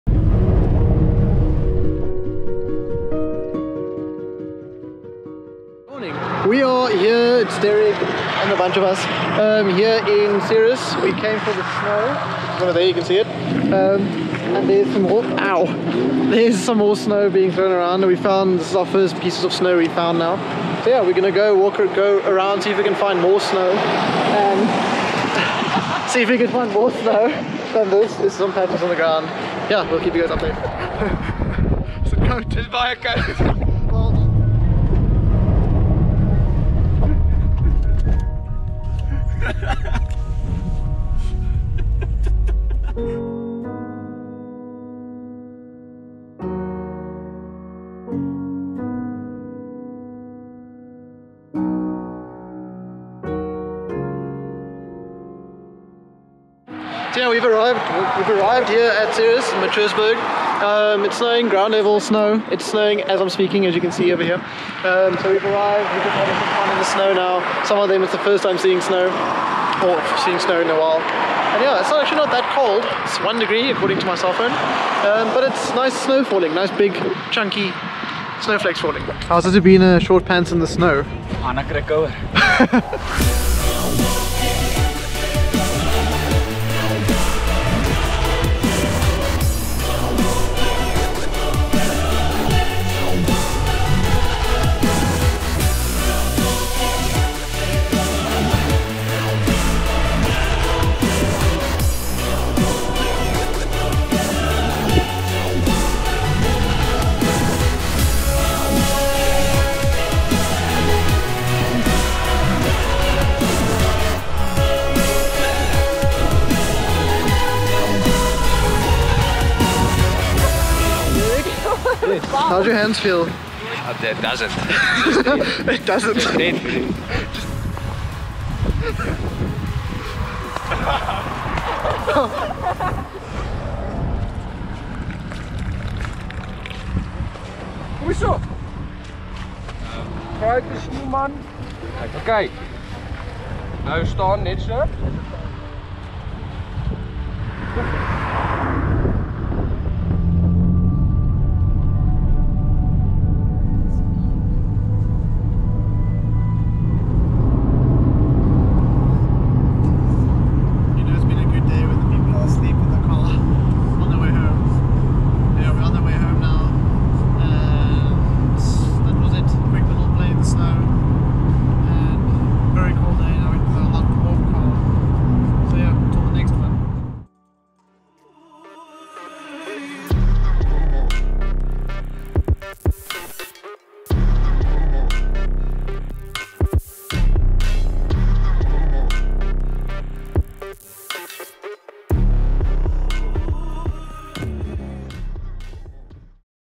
Ceres Snow Mission | Searching for snow in South Africa!
As spring closes in the winter gets colder. A last-minute decision made us all head out to Ceres in search of snow and our luck was in this time. We arrived just outside Matroosberge and it started snowing. A few snowball fights later and we called it a day. Plus we couldn't feel our hands anymore!